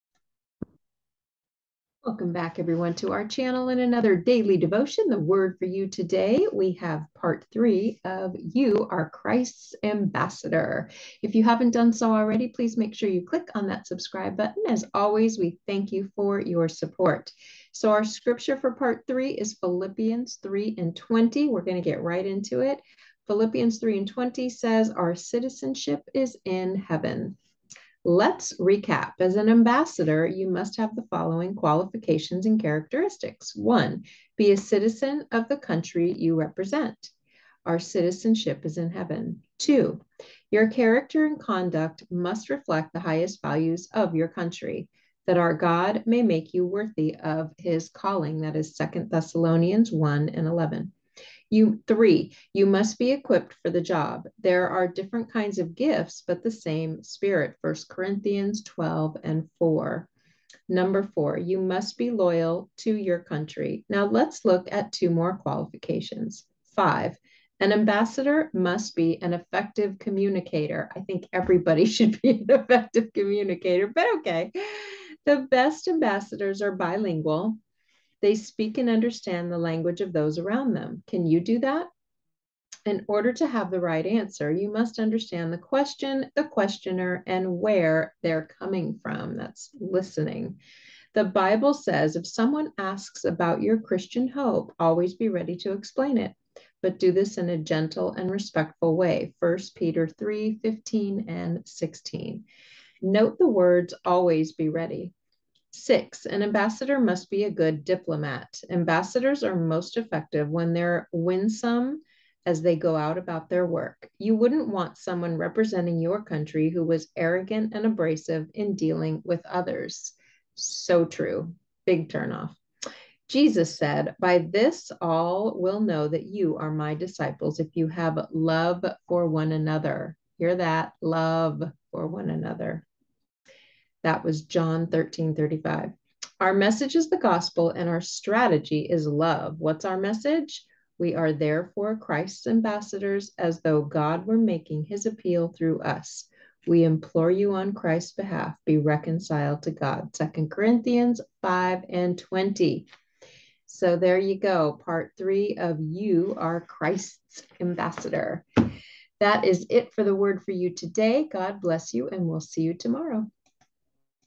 Daily Devotion: "You Are Christ's Ambassador" (3)
The Word for You Today. Scripture for this devotion is Philippians 3:20 "Our citizenship is in heaven."